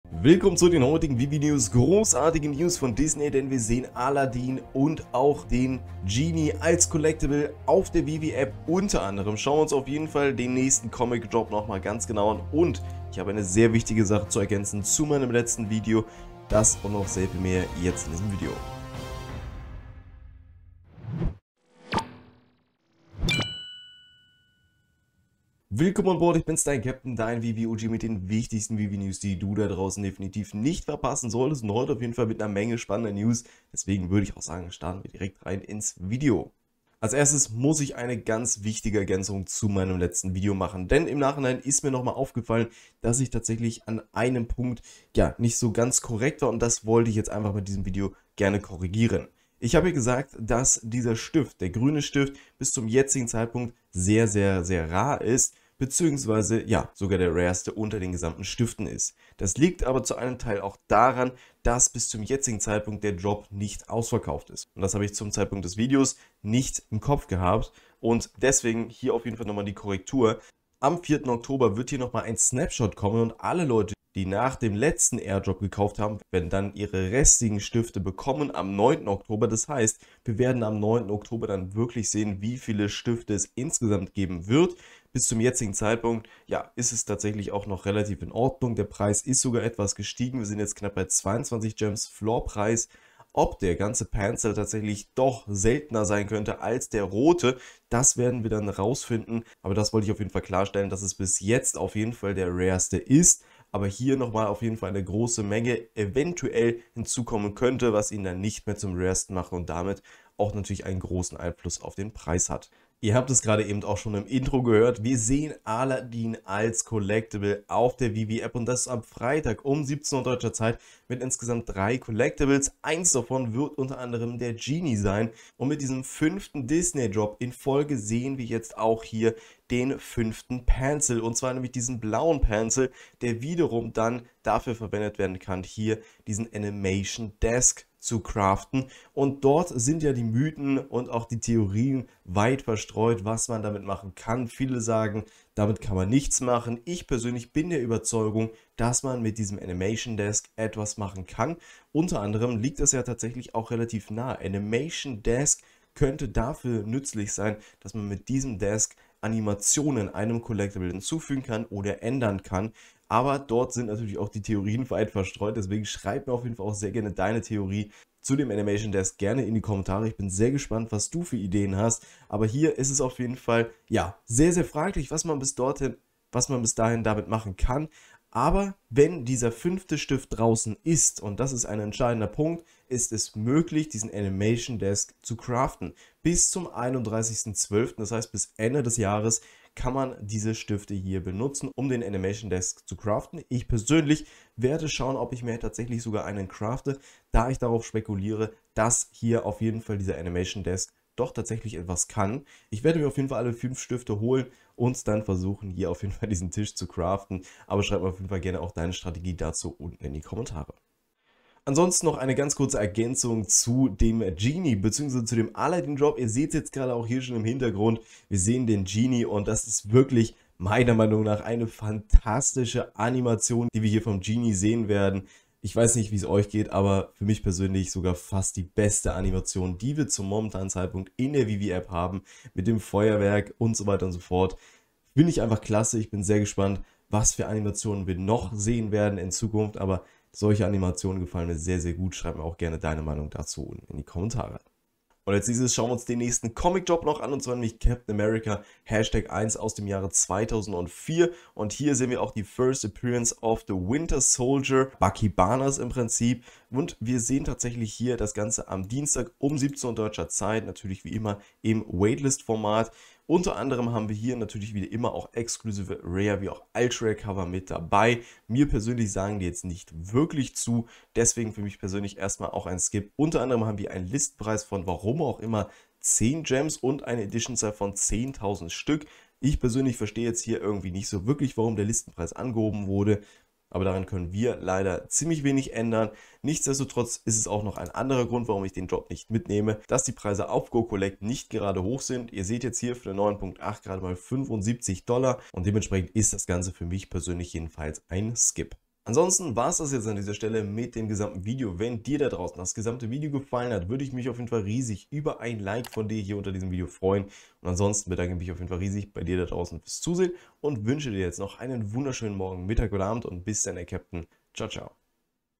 Disney Aladdin + Ginie NFT Drop! + wichtiger Hinweis! | VeVe deutsch
↪️ Dsiney bringt Aladin und den Ginie als NFT auf die VeVe App und ich gebe einen wichtigen Hinweis zu meinem letzten Video.